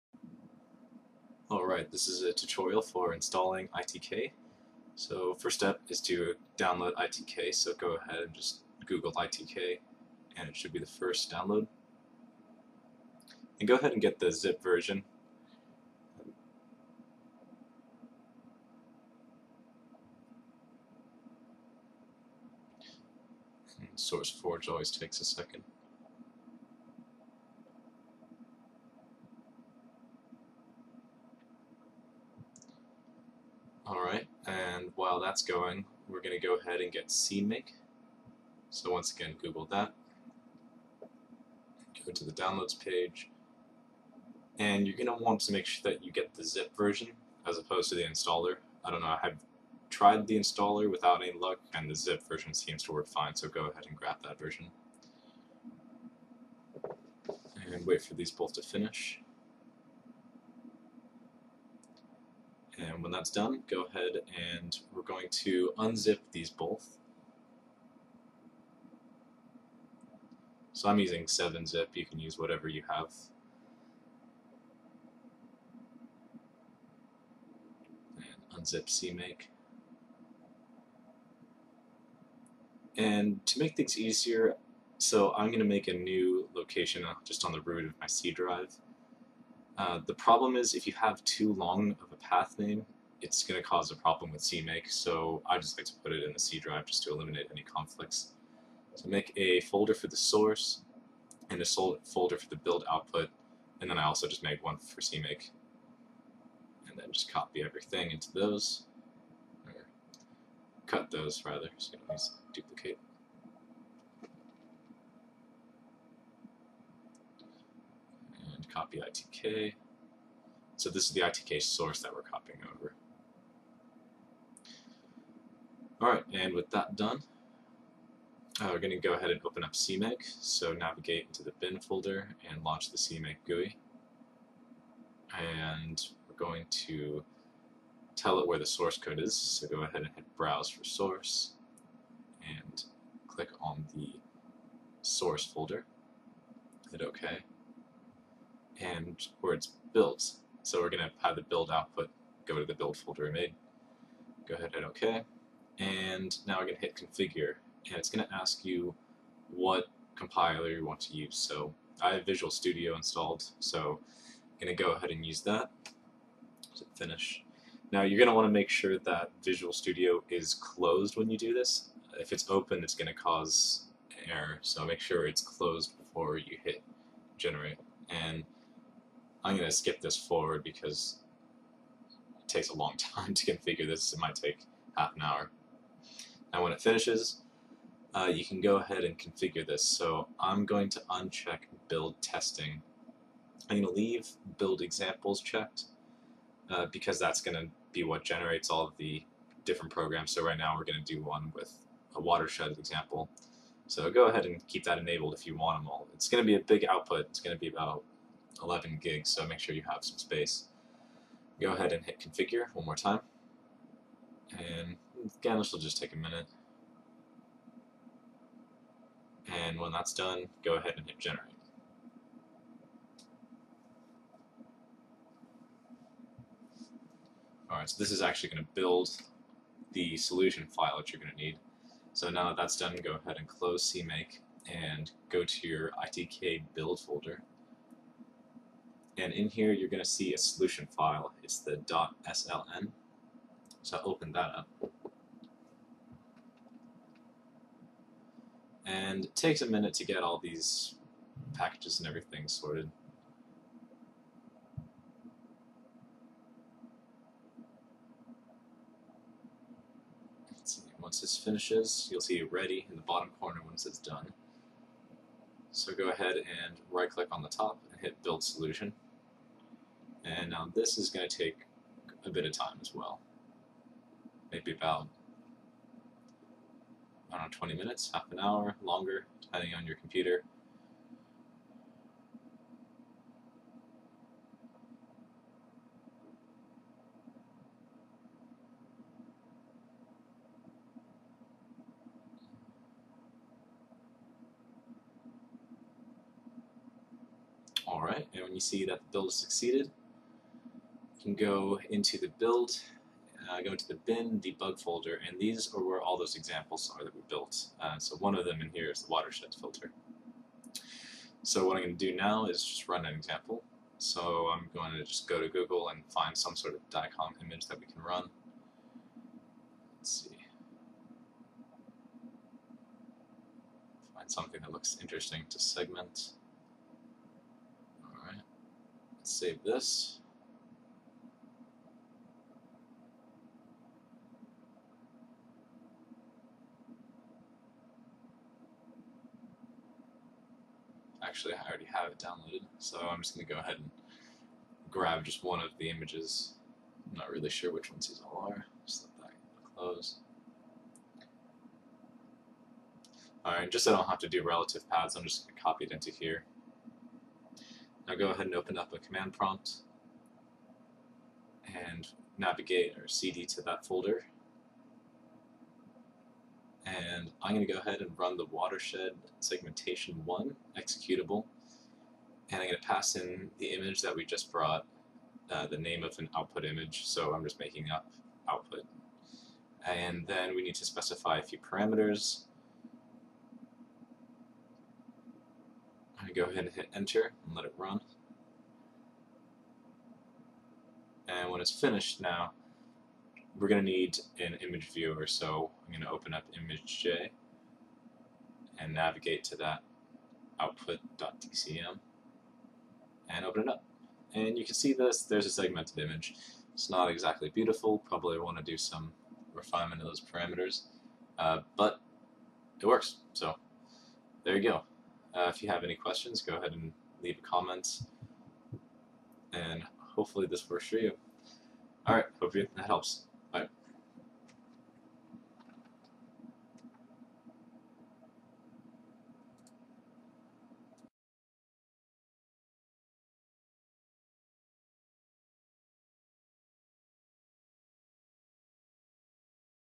ITK Install Tutorial
Tutorial video on how to install ITK and run an sample using the watershed segmentation example.

My system is pretty powerful (quad core, 8GB RAM, SSD) and it still took me about an hour to build all this. Expect that less powerful systems or laptops with slow disk I/O might take quite a bit longer.